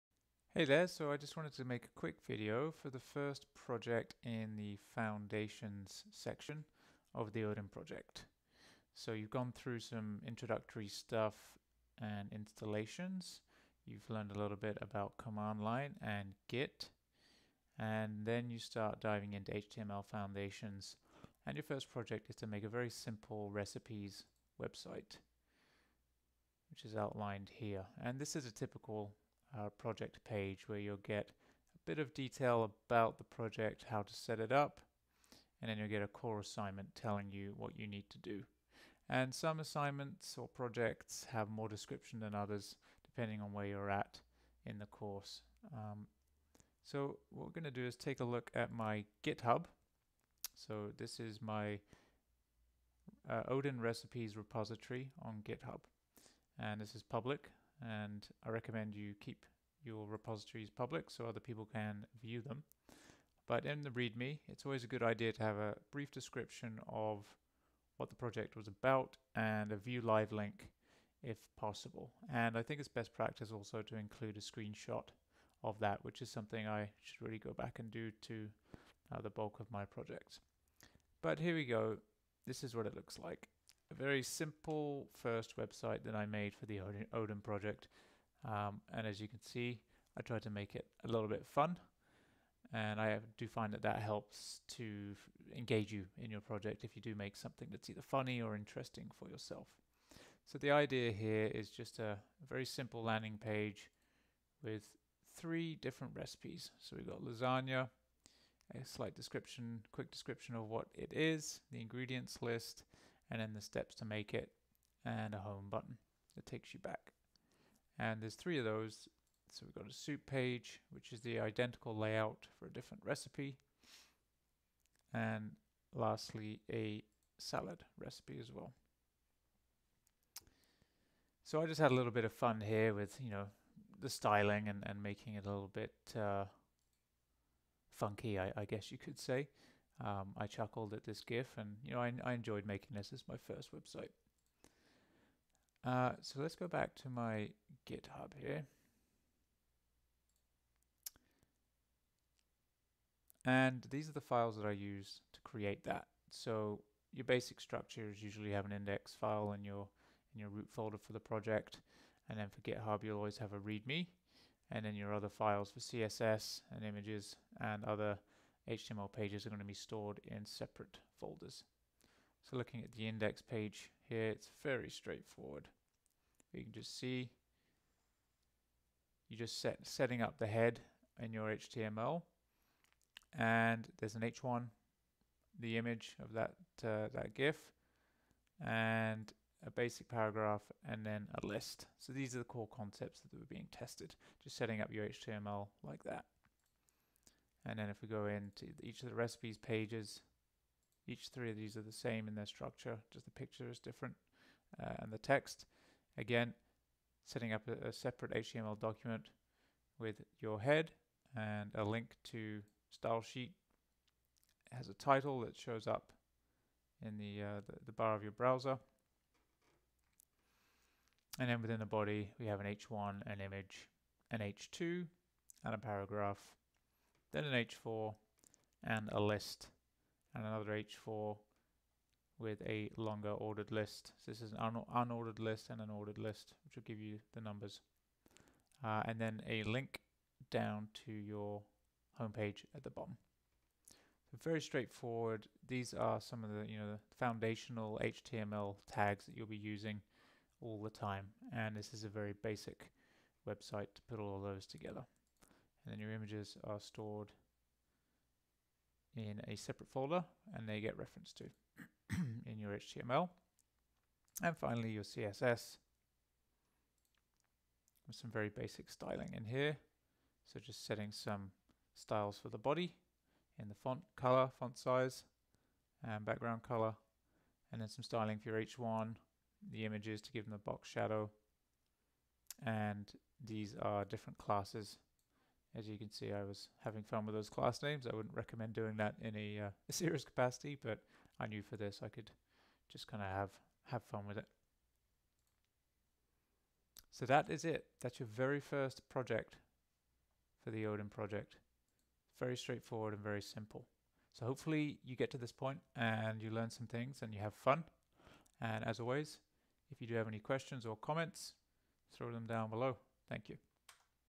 The Odin Project - Foundations Recipes Page
Foundations Project # 1

This is a video showing you my first project in the Foundations section of The Odin Project - a simple webpage with basic HTML and CSS.